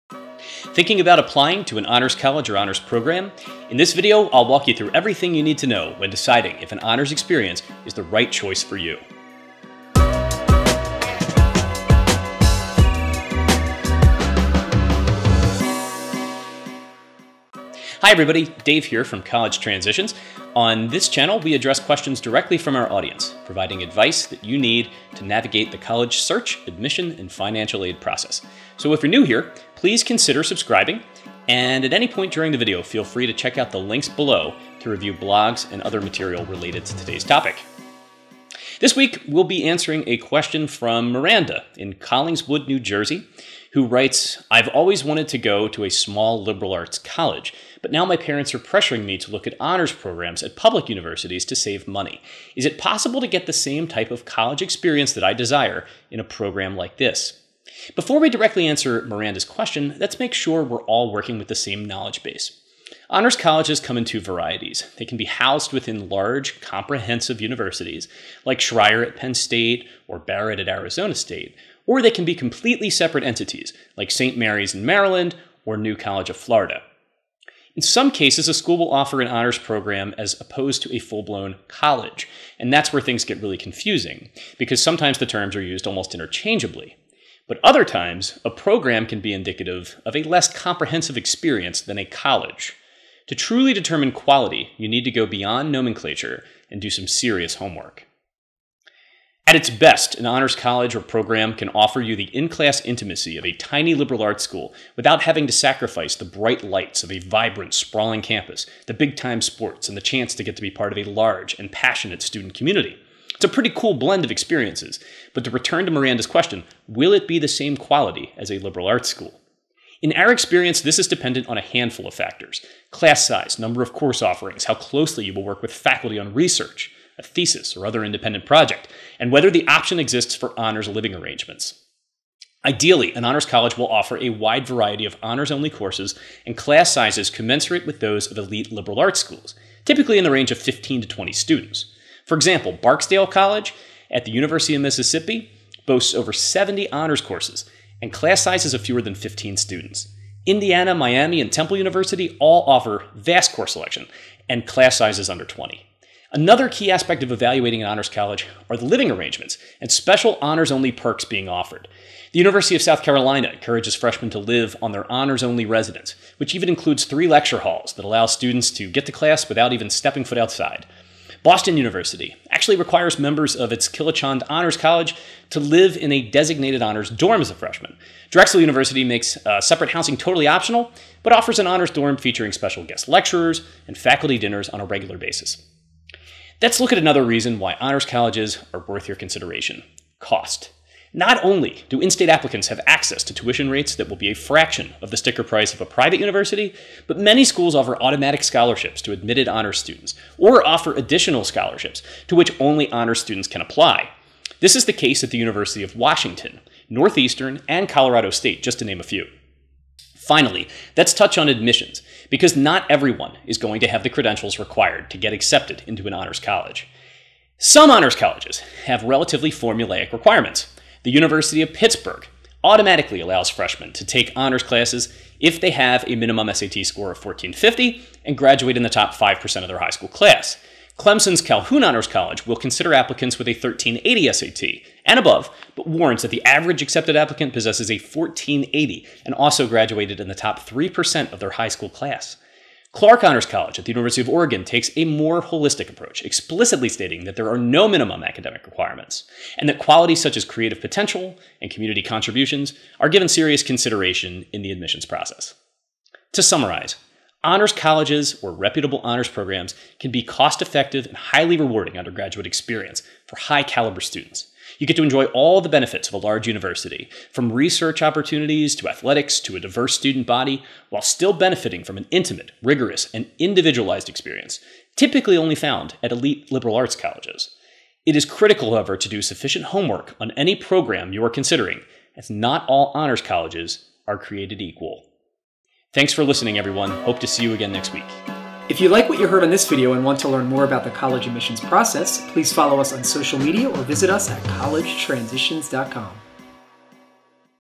Honors Colleges: What you need to know
In this video, Dave Bergman of College Transitions provides insight into what students (and their parents) need to consider when evaluating honors colleges and programs.

📘 Colleges Worth Your Money
📗 The Enlightened College Applicant